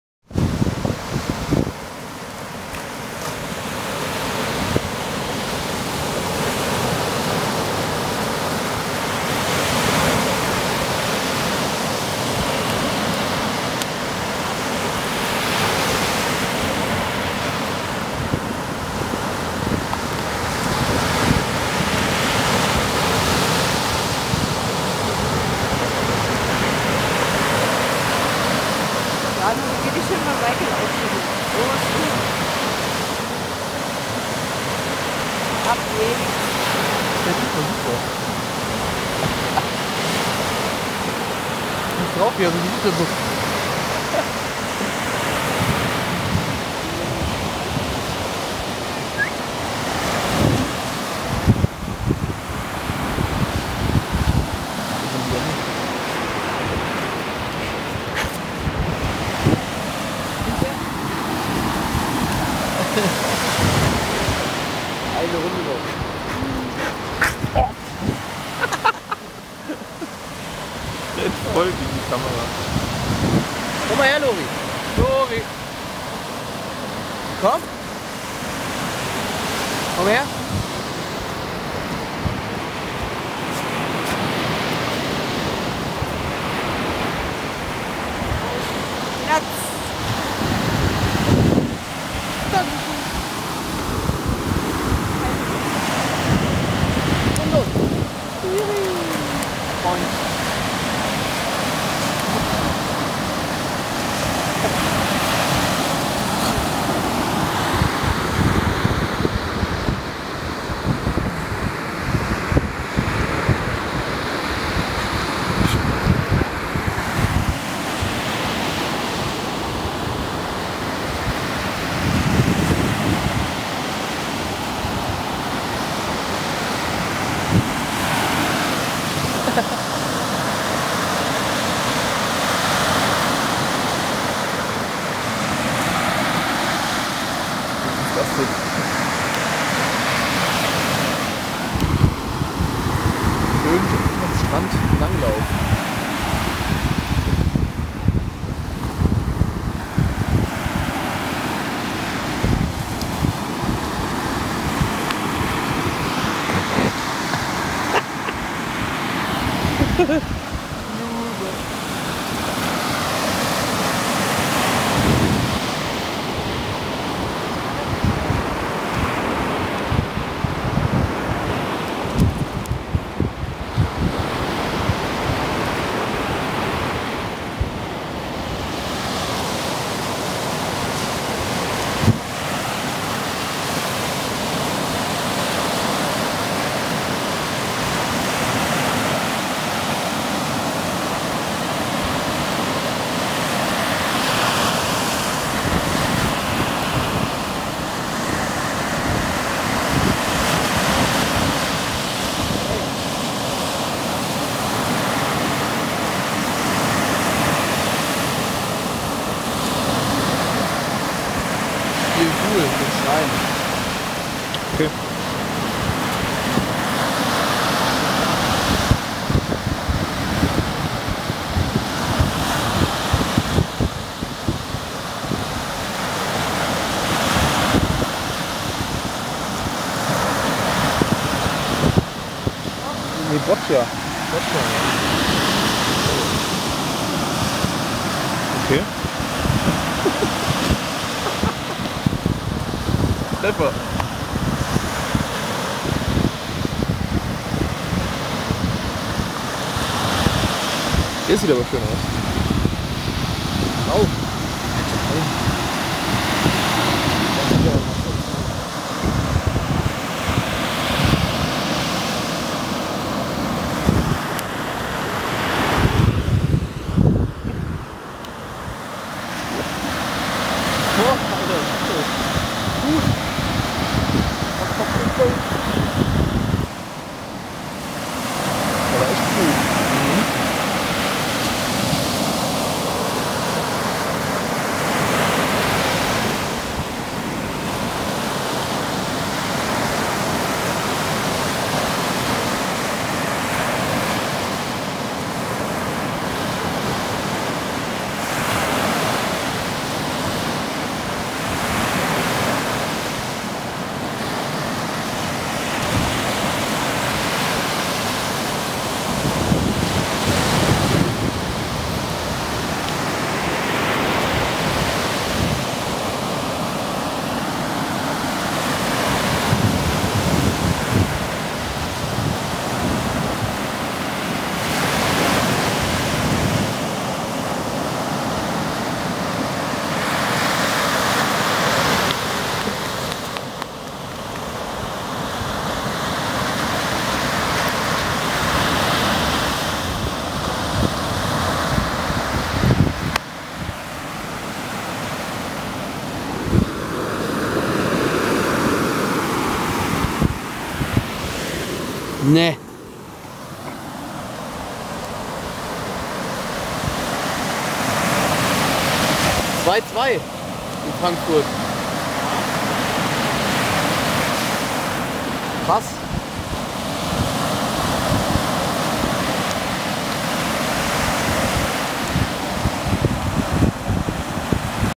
Ein chilliger Tag an der Ostsee - Part 2
Chilliger Tag an der Ostsee mit Lory, dem verrückten Hund  ;)

Teil 2